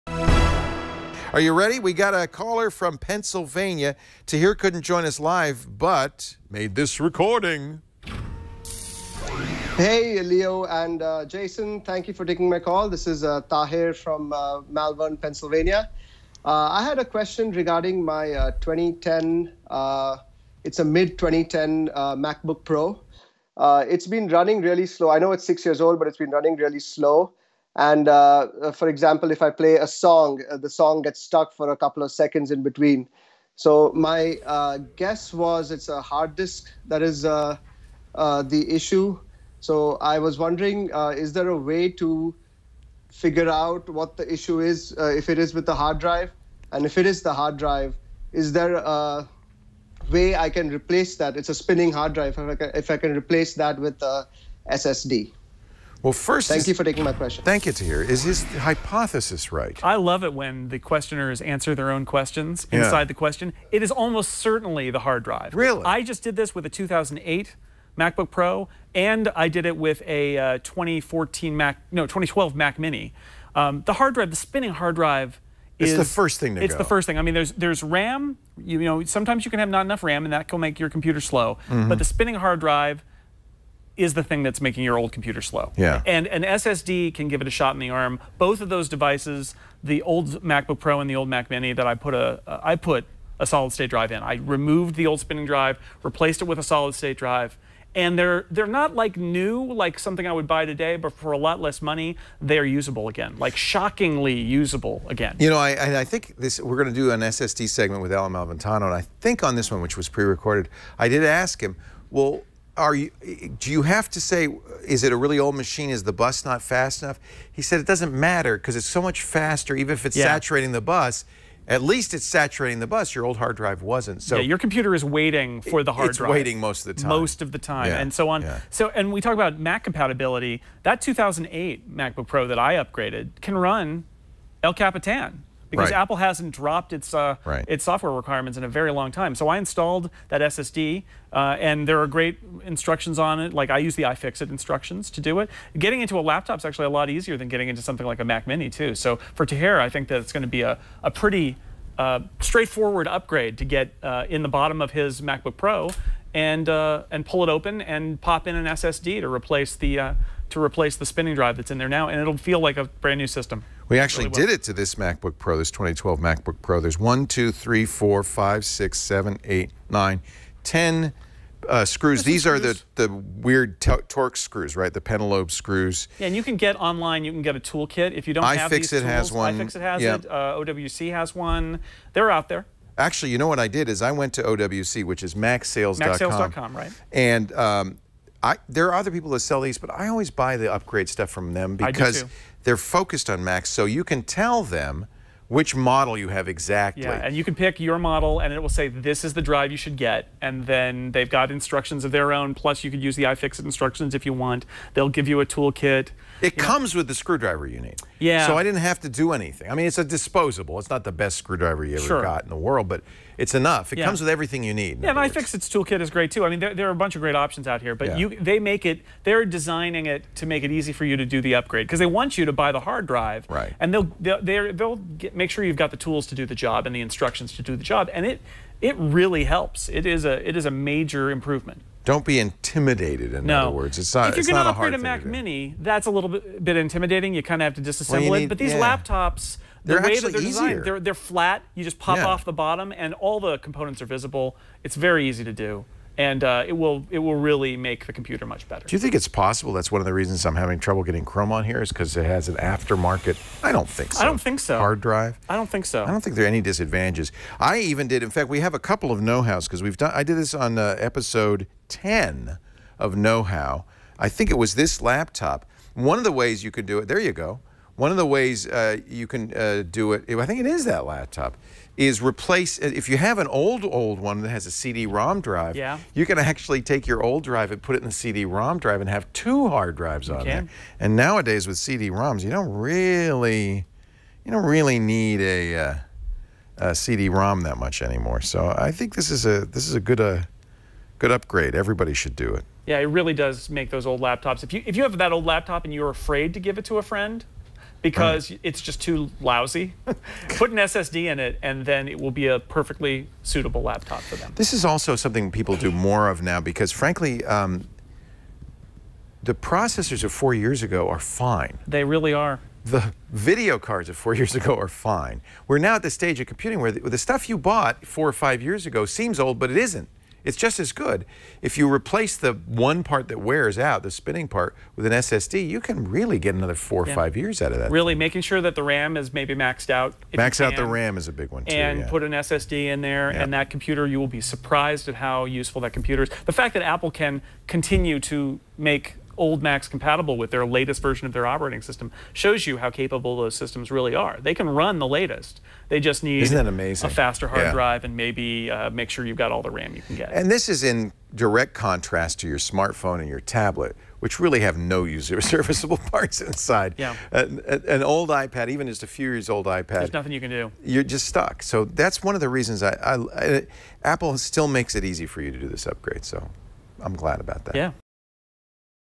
Replace HDD with SSD in a MacBook Pro
Leo Laporte and Jason Snell help a viewer replace their spinning hard drive in a MacBook Pro with an SSD.